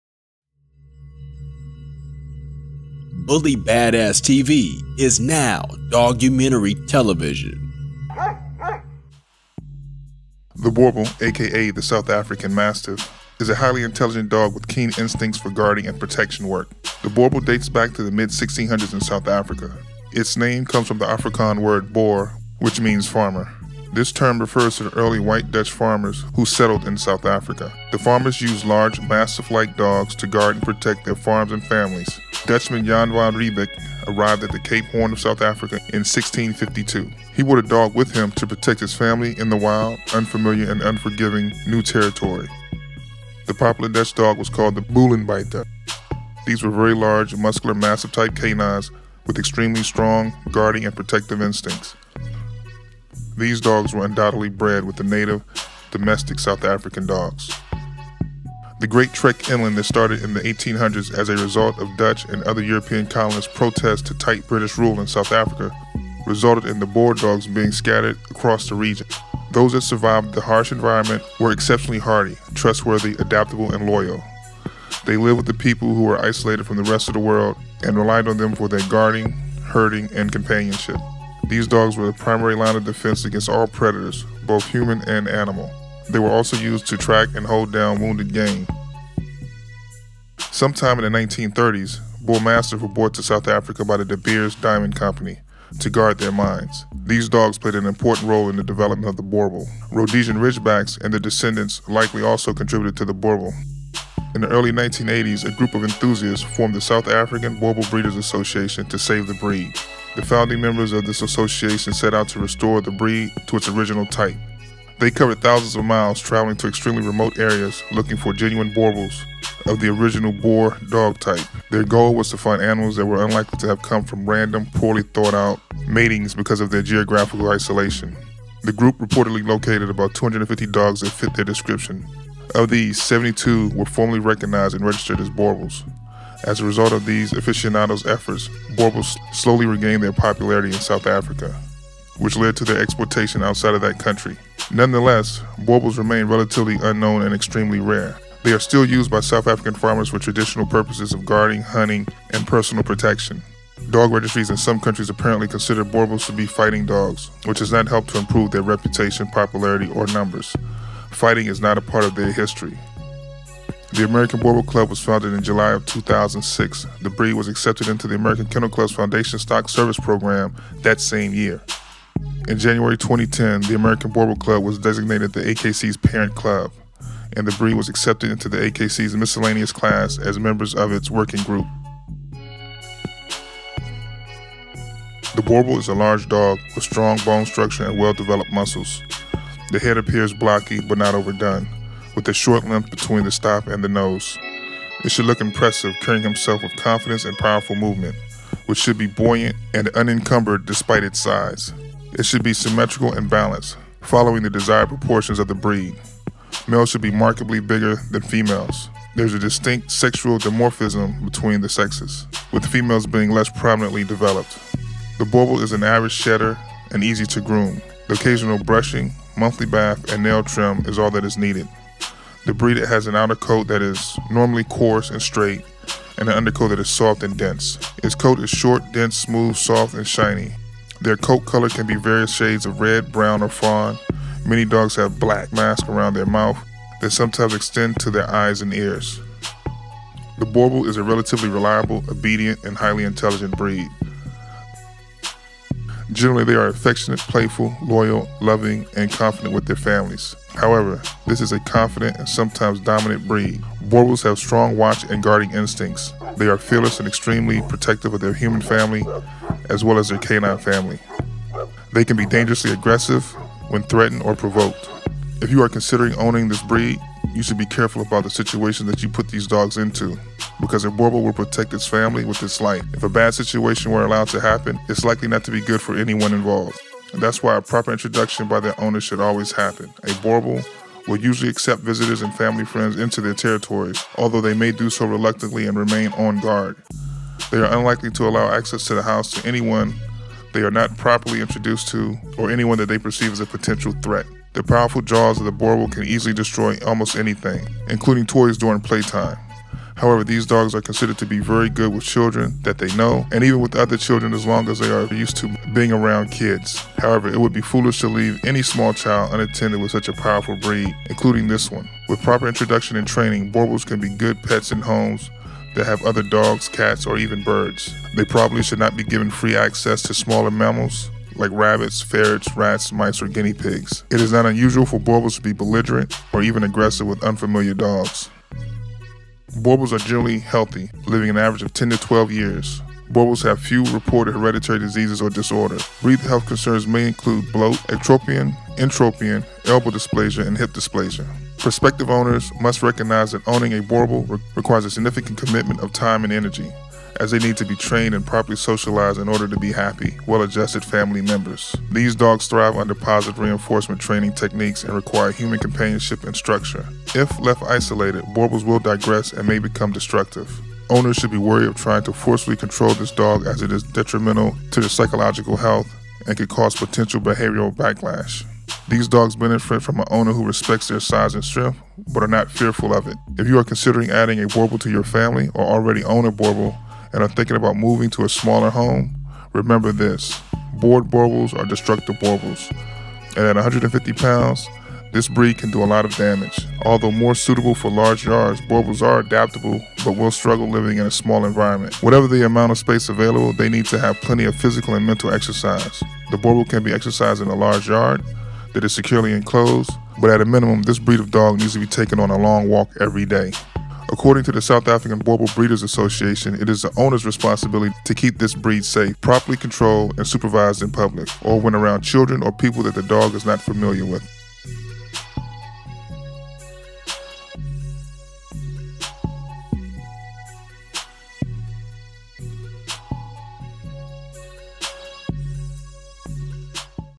BOERBOEL: A DOG LOVER'S INTRODUCTION
This is a dog lover's introduction into one of if not my favorite breed. The boerboel aka the South African Mastiff is a highly intelligent dog with keen instincts for guarding and protection work.
The Boerboel Breed History
The Boerboel dates back to the mid-1600s in South Africa. Its name comes from the Afrikaan word “boer,” which means “farmer.” This word refers to the early white Dutch farmers who settled in South Africa. The farmers used large, Mastiff-like dogs to guard and protect their farms and families. Dutchman Jan van Riebeeck arrived at the Cape Horn of South Africa in 1652. He brought a dog with him to protect his family in the wild, unfamiliar and unforgiving new territory. That popular Dutch dog was called a "Bullenbitjer." These were large, muscular Mastiff-type canines with extremely strong guarding and protective instincts. These dogs undoubtedly were bred with native domestic South African dogs.
The Great Trek inland that started in the 1800s, as a result of Dutch and other European colonists protest to tight British rule in southern Africa. Resulted in the Boer dogs being scattered across the region. Those that survived the harsh environment were exceptionally hardy, trustworthy, adaptable and loyal. They lived with people who were isolated from the rest of the world and relied on them for guarding, herding and companionship. These dogs were a primary line of defense against all predators, both wild and human. They also were used to track and hold down wounded game, until the hunters could retrieve it and use it as a source of food.
Sometime in the 1930s, Bullmastiffs were brought to South Africa by the De Beers diamond company to guard their mines. These dogs played an important role in the development of the Boerboel. Rhodesian Ridgebacks and their descendants likely contributed to the Boerboel as well.